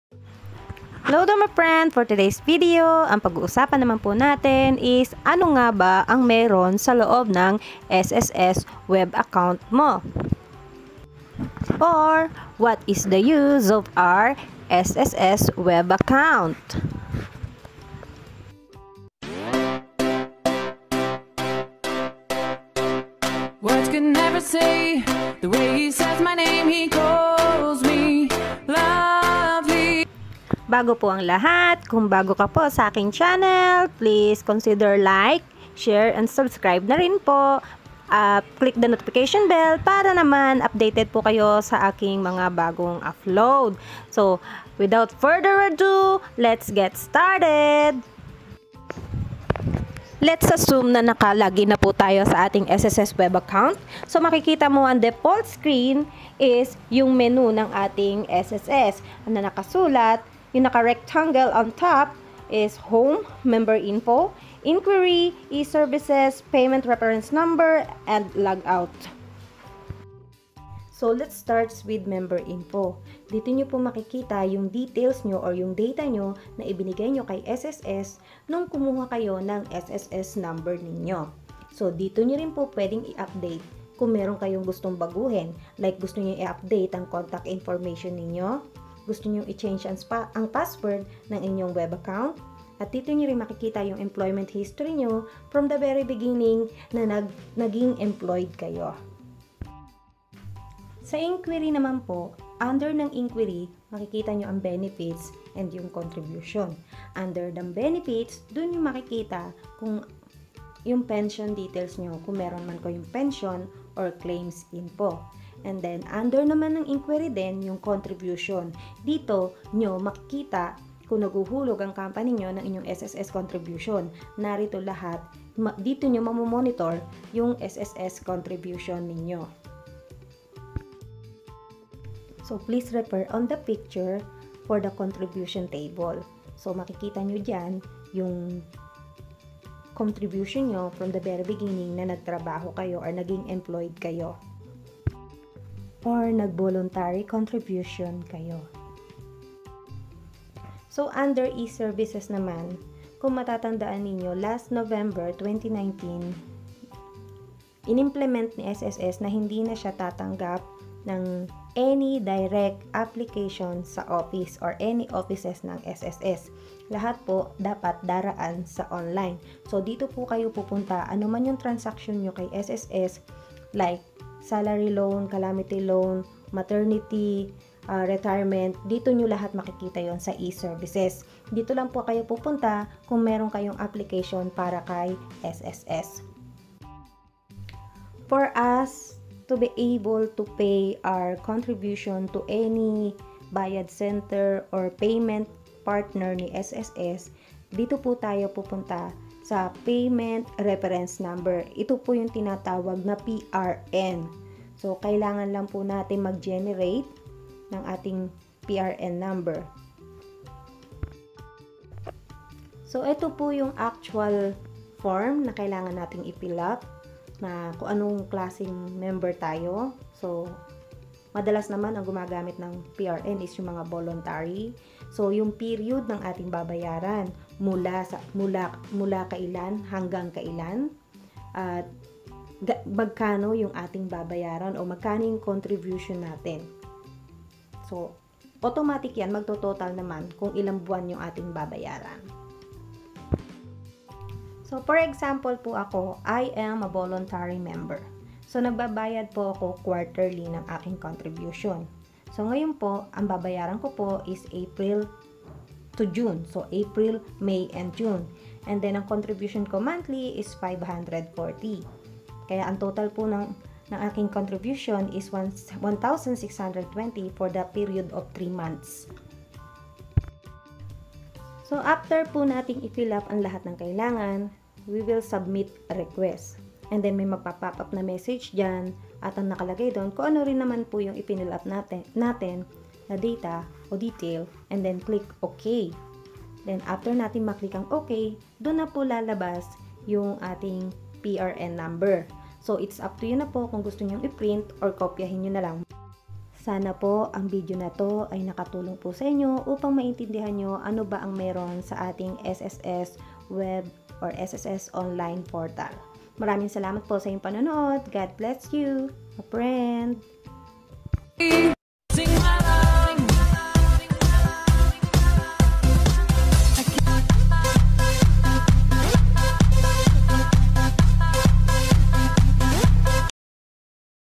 Paano gamitin ang SSS ONLINE ACCOUNT?
In this video we will be discussing about What is the USE of SSS WEB ONLINE PORTAL or what is inside the PORTAL, although many of the members already had online account, still many kept asking how to used it, I hope this simple tutorial can be a help!